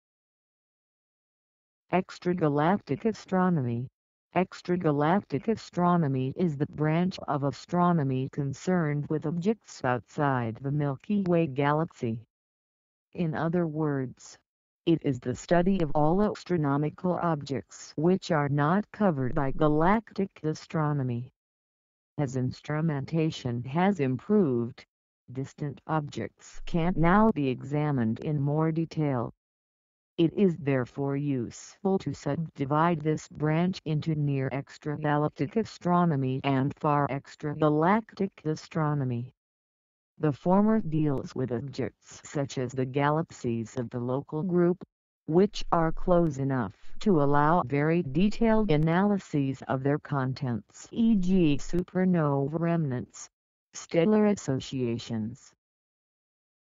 What Is EXTRAGALACTIC ASTRONOMY? EXTRAGALACTIC ASTRONOMY Definition & Meaning
What is EXTRAGALACTIC ASTRONOMY, What does EXTRAGALACTIC ASTRONOMY mean, EXTRAGALACTIC ASTRONOMY meaning, EXTRAGALACTIC ASTRONOMY definition, EXTRAGALACTIC ASTRONOMY explanation

Extragalactic astronomy is the branch of astronomy concerned with objects outside the Milky Way galaxy. In other words, it is the study of all astronomical objects which are not covered by galactic astronomy.

As instrumentation has improved, distant objects can now be examined in more detail. It is therefore useful to sub-divide this branch into Near-Extragalactic Astronomy and Far-Extragalactic Astronomy. The former deals with objects such as the galaxies of the Local Group, which are close enough to allow very detailed analyses of their contents (e.g. supernova remnants, stellar associations).
Source: Wikipedia.org